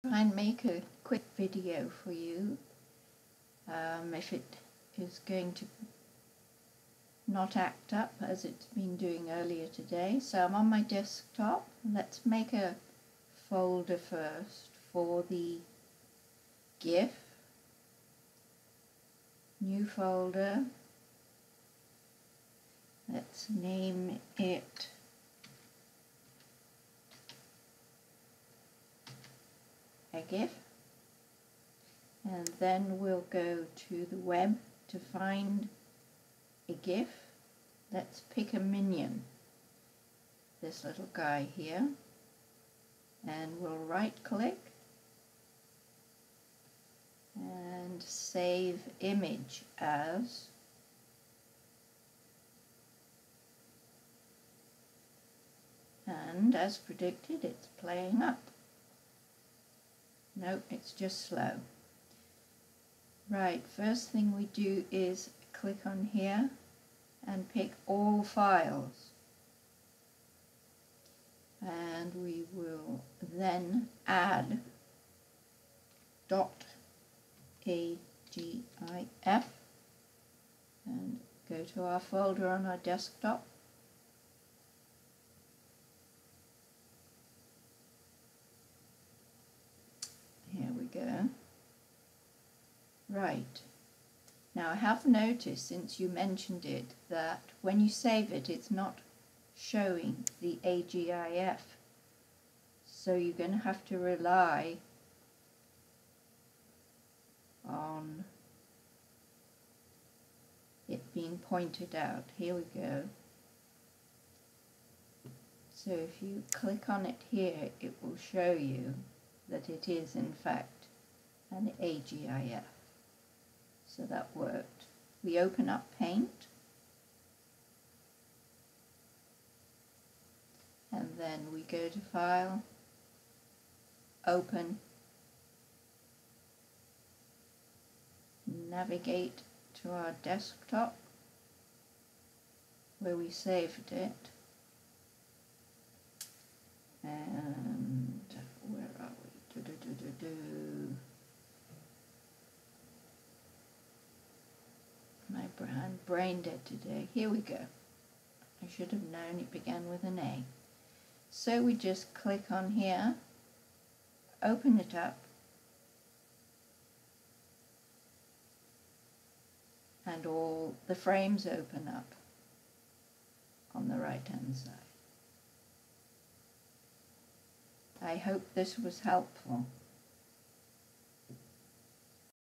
How to open layers in a Gif into paint.net.
How to import a Gif into paint.net using the AGIF Plugin in paint.net.